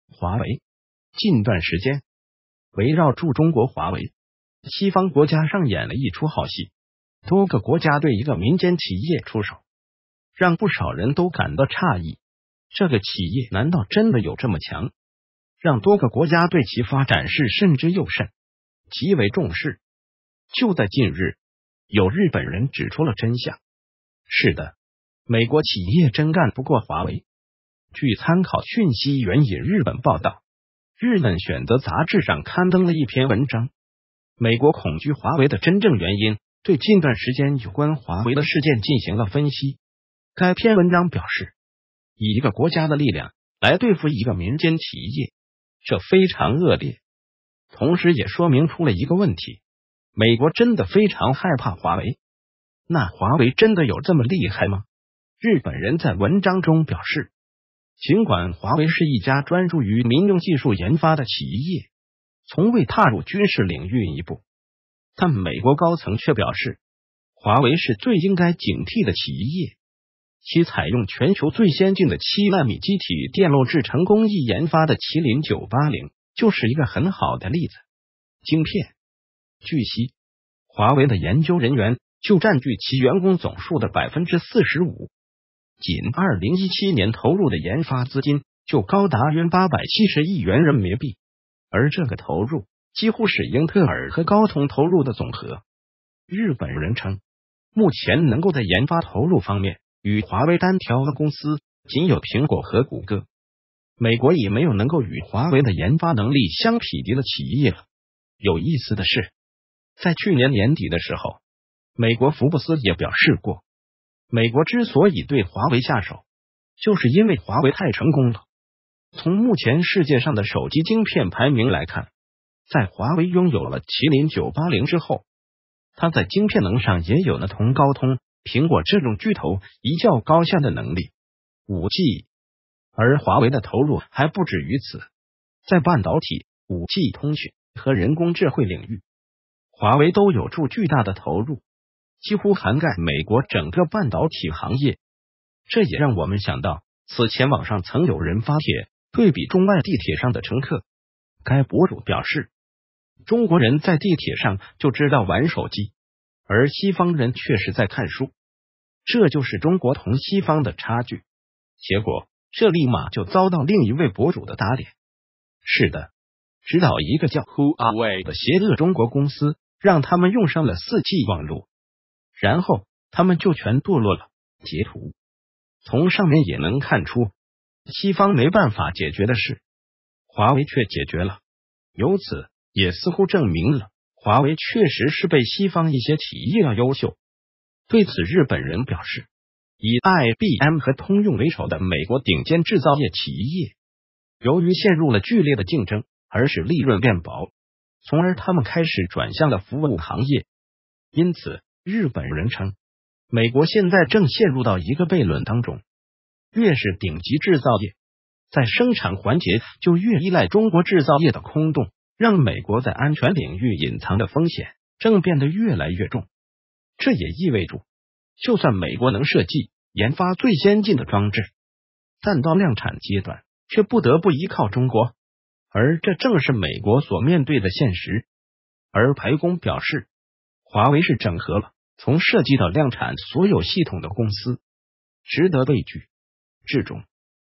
華為真的有這麼厲害嗎？外國人分析：其研發能力，無美企可匹敵！
華為近段時間，圍繞著中國華為，西方國家上演了一出好戲。就在近日，有日本人指出了真相，“是的，美國企業真幹不過華為”。